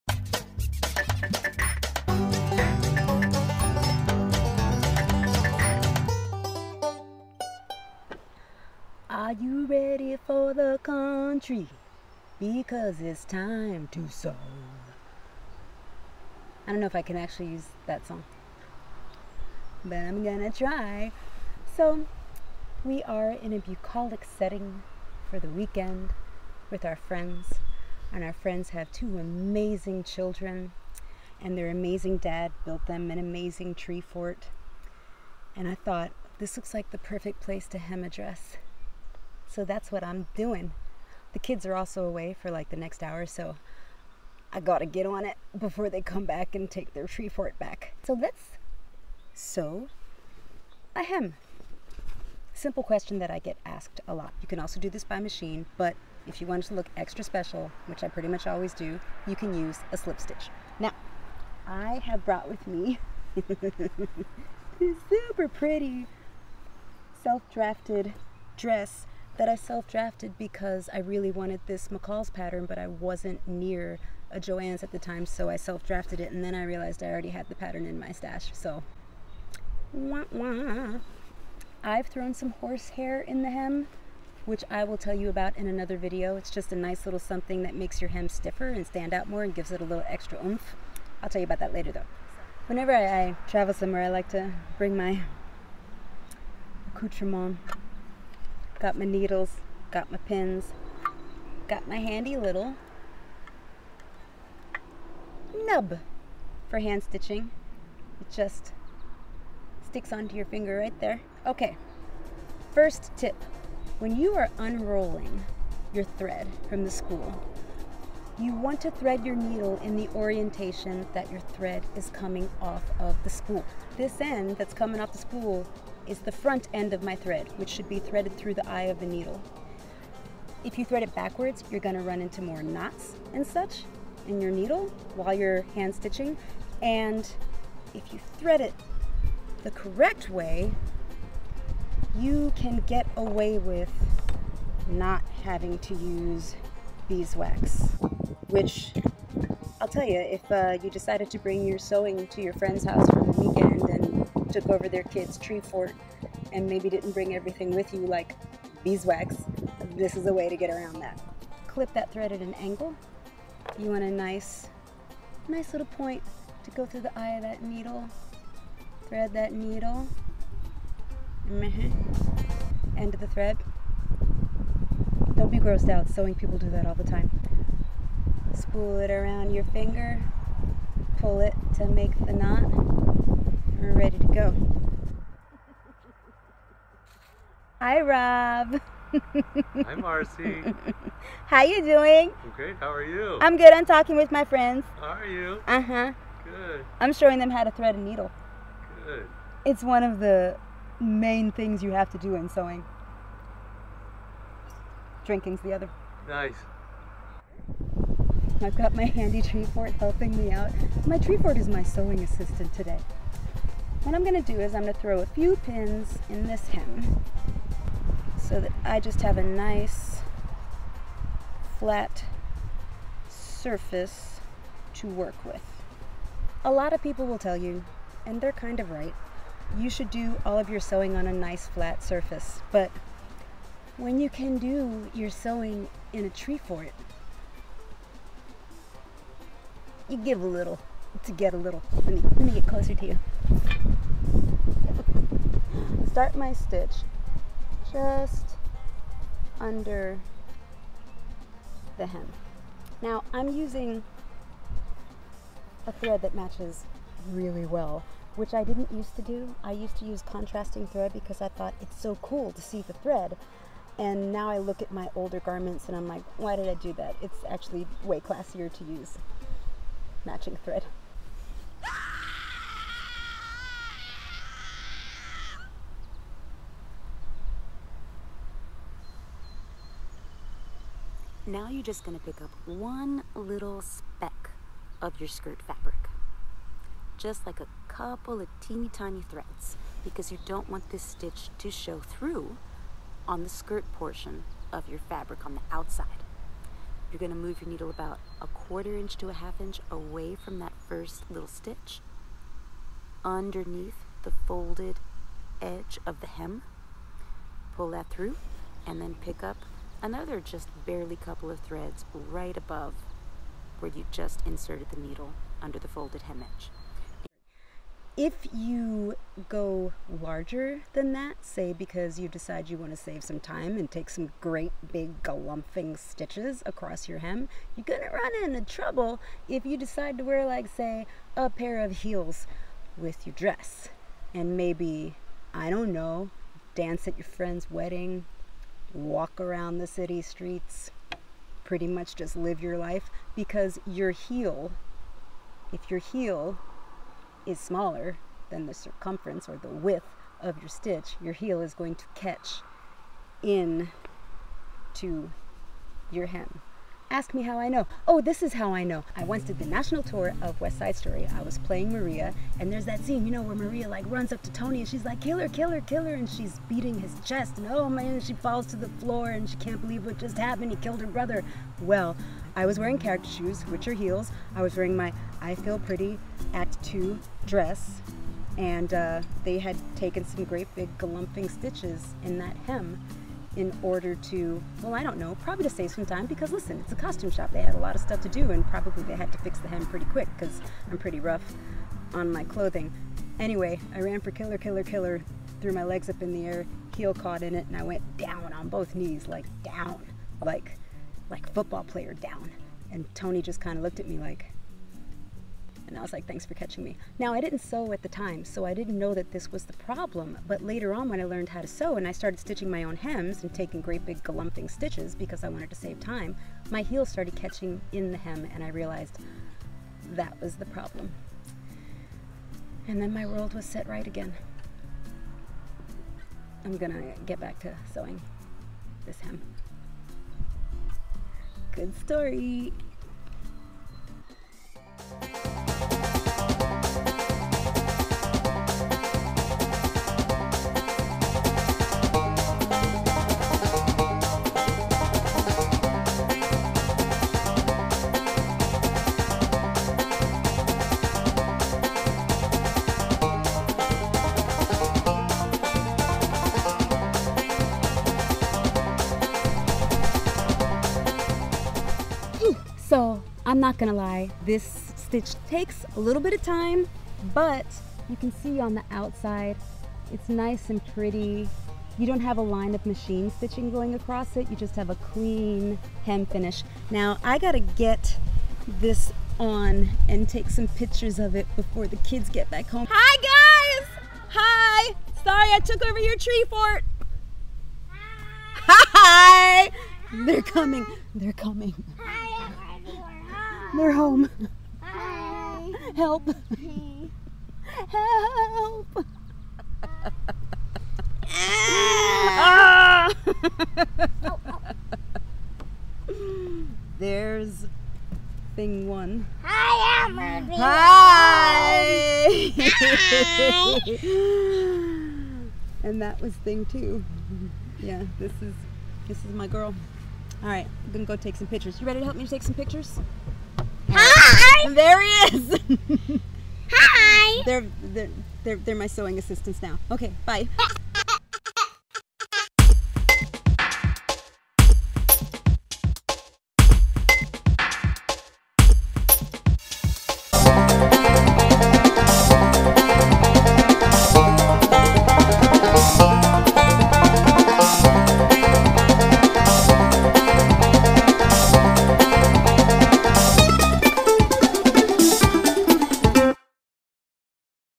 Sewing in the Wild...How to Hand Sew a Hem!
Wanna learn how to hand sew a hem, on just about anything? I'll show you. But more importantly, I'll take over a toddler's tree fort, tell a random story or two, and terrify you with my lack of a manicure.

SUPPLIES USED:

✸Thimble Pack (aka my "nub")
✸Gingher Thread Snips (springloaded!)
✸Gutterman Mara Thread 100
✸Silk Pins

I'M WEARING:

✸True Bias Ogden Cami Pattern (hacked to a tiered dress)
✸Self-drafted cotton dress (try McCalls 7949 to get the look!  )

MORE DRESS SHOTS & MORE SEWING!

❤️thanks audionautix!